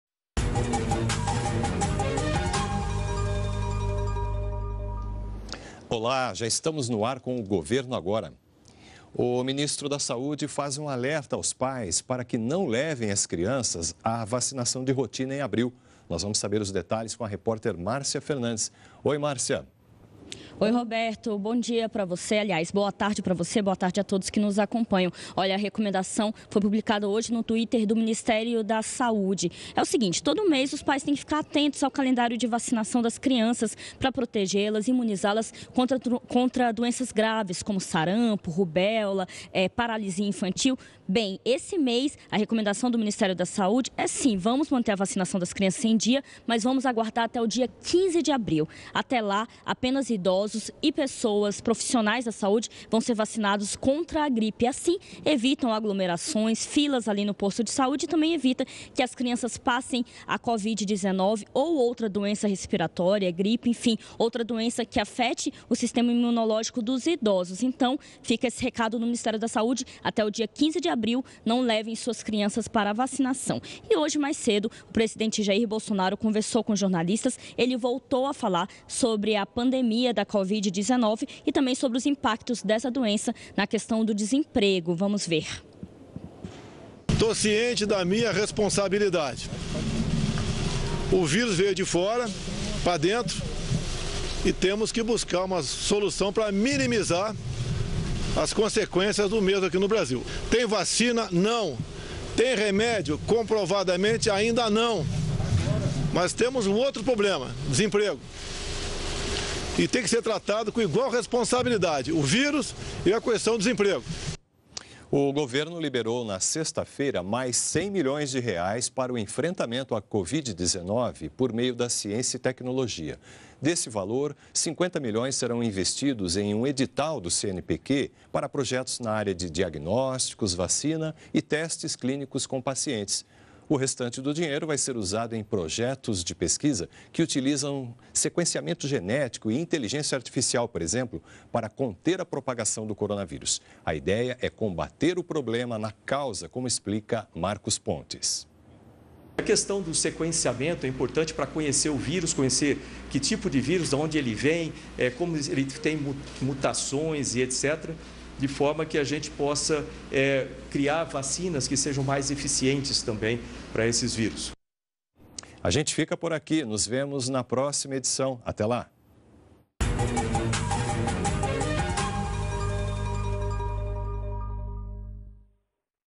Pais devem evitar levar crianças para vacinar em abril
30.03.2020 - O ministro da Saúde faz um alerta aos pais para que não levem as crianças para vacinação de rotina em abril; o governo liberou na sexta-feira mais R$ 100 mi para o enfrentamento à Covid-19 por meio da ciência e tecnologia.